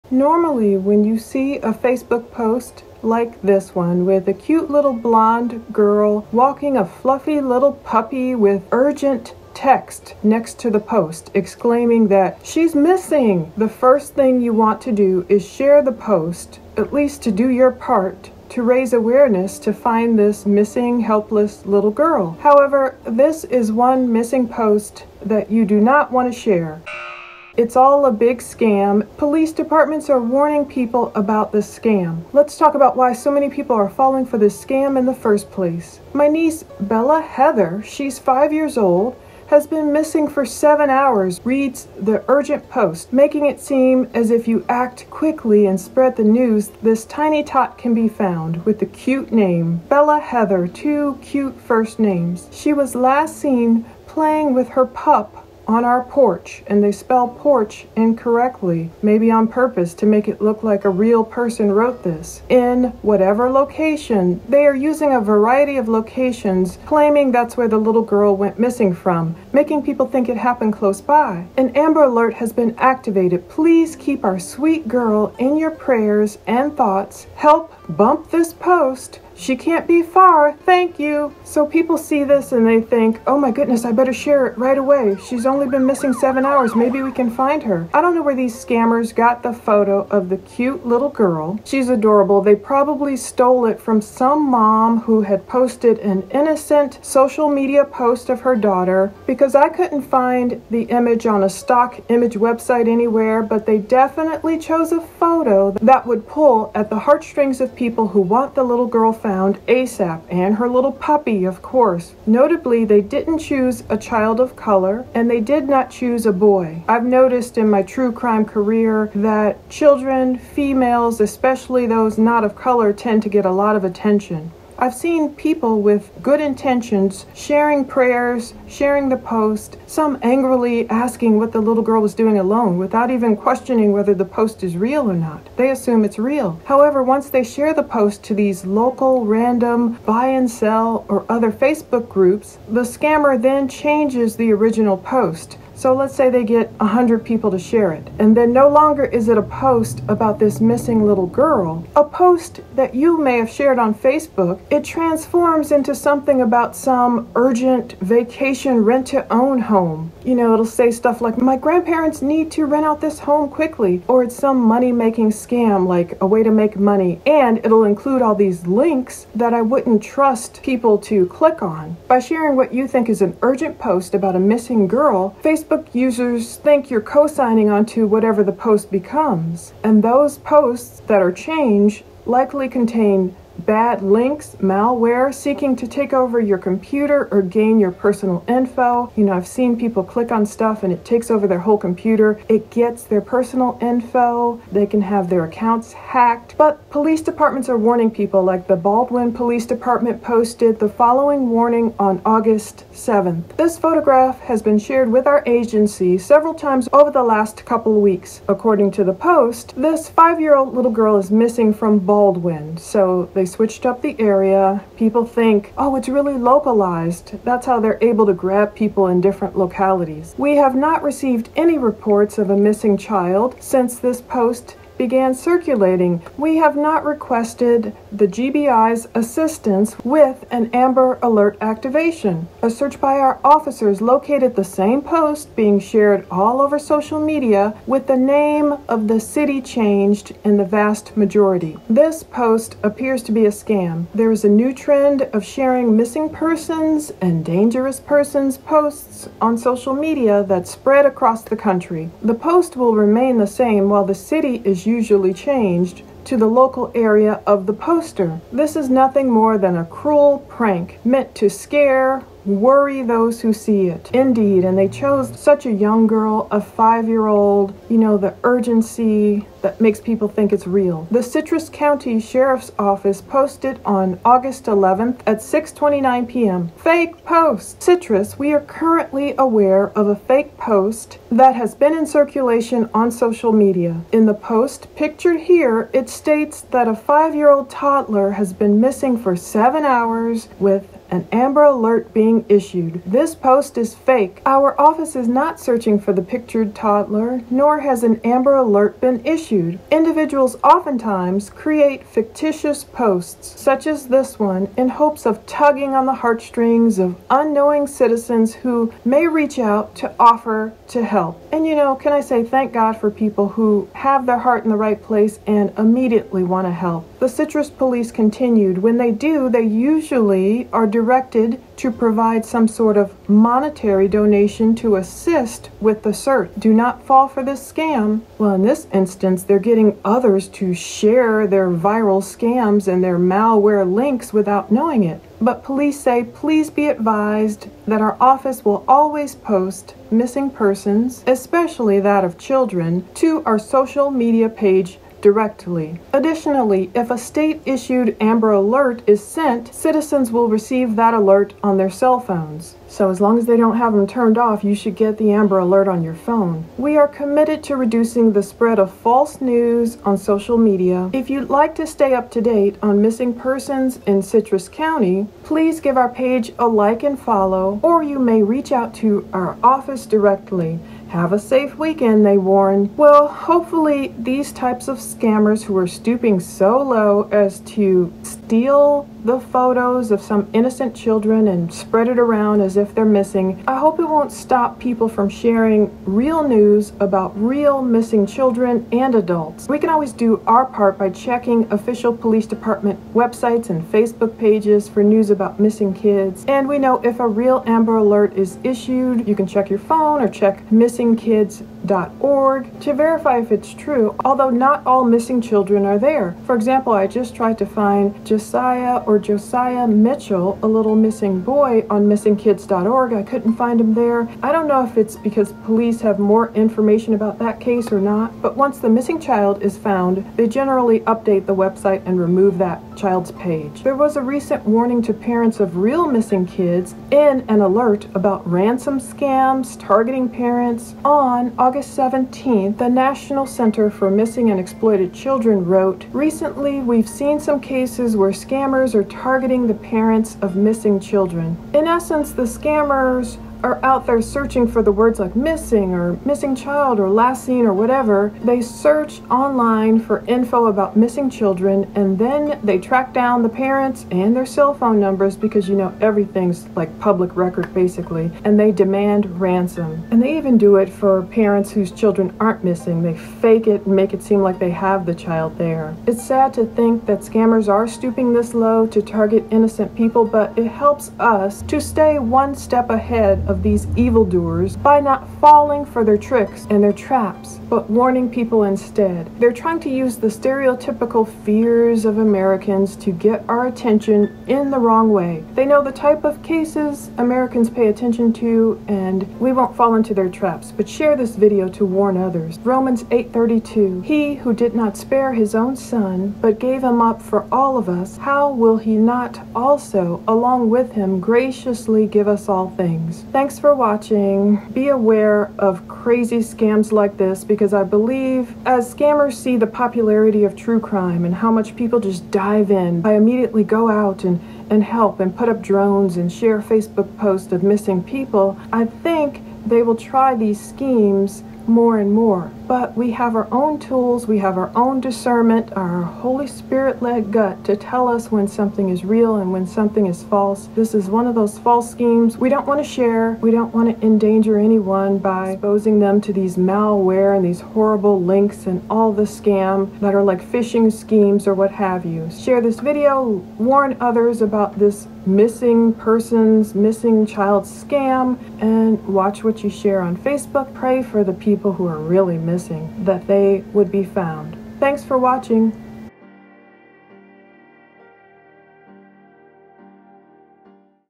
Why Bella Heather, 5, is the PERFECT missing child scam: Girl has no Amber Alert but photo is viral
If you would like to watch this video with NO ADS, watch it on my SPOTIFY PLUNDER TRUE CRIME

Normally, when you see a Facebook post like this one, with a cute little blonde girl walking a fluffy little puppy with urgent text exclaiming that she's !!MISSING!! - the first thing you want to do is share it to raise awareness.

However, this is one missing post that you don't want to share. It's all a big scam and police departments are warning people about it—but let’s talk about why so many people are falling for it.

"My niece Bella Heather she's 5years old has been missing for 7hours," reads the urgent post, making it seem as if you act quickly and spread the news, this tiny tot can be found.

"She was last seen playing with her pup on our pourch [with porch spelled incorrectly] in [whatever location, a variety of locations are being claimed, which makes people think it’s happening close by.]

"An amber alert has been activated. Please keep our sweet girl in your prayers and thoughts help bump this post. She can't be far. Thank you❤"

I don't know where these scammers got the photo of the cute little girl--they probably stole it from some mom who posted an innocent social media post of her daughter, because I couldn't find it on a stock image website, but they definitely chose a photo that would pull at the heartstrings of people who want her found.

Notably, they didn’t choose a child of color. Nor did they choose a boy.

Indeed, I’ve seen well-meaning people sharing the post with prayers she’ll be found, or angrily asking what the little girl was doing alone.

however, once the post is shared to these local random "buy and sell" or other Facebook groups, the scammer changes the original post.

No longer is it a post about a missing girl, the post that you may have shared on Facebook transforms into something about a vacant rent-to-own home or a money-making scam with links I wouldn't trust to click.

By sharing what you think is a missing girl's urgent post, Facebook users think you're cosigning whatever scam they change the post to reflect.

And those posts likely contain bad links, malware, that seeks to take over your computer or gain your personal info.

POLICE DEPTS ARE PUTTING OUT THE WARNING:

The Baldwin Police Department posted the following warning on August 7th:

This photograph has been shared with our agency several times over the last couple weeks.

According to the posts, this 5 year old little girl is missing from Baldwin.

We have not received any reports of a missing child since this post began circulating.

We have not requested the GBI’s assistance with an Amber Alert activation.

A search by our officers located the same post being shared all over social media with the name of the city changed in the vast majority.

This post appears to be a scam. There is a new trend of sharing missing persons and dangerous persons posts on social media that spread across the country.

The post will remain the same while the city is usually changed to the local area of the poster. This is nothing more than a cruel prank meant to scare/worry those who see it.

Citrus County Sheriff's Office POSTED ON
August 11 at 6:29 PM
🚨 FAKE POST ‼️ - Citrus, we are currently aware of a fake post that has been in circulation on social media. In the post pictured here, it states that a five-year old toddler has been missing for seven hours, with an Amber Alert being issued.

This post is FAKE. Our office is not searching for the pictured toddler, nor has an Amber alert been issued.

Individuals often times create fictitious posts, such as this one, in hopes of tugging on the heartstrings of unknowing citizens who may reach out to offer help.

When they do, they usually are directed to provide some sort of monetary donation to “assist” with the search.  Do not fall for this SCAM.

Additionally, if a state issued Amber Alert is sent, citizens will receive that alert on their cell phones.

We are committed to reducing the spread of false news on social media. If you’d like to stay up-to-date on missing persons in Citrus County, please give our page a like and follow, or you may reach out to our office directly.
Have a safe weekend!

Hopefully these scammers won't stop people from sharing the news about real missing children.

We can always check official police department websites and Facebook pages for news about missing kids.

In essence, the scammer search for info about missing children then track down the parents cellphone numbers and demand ransom.

It's sad.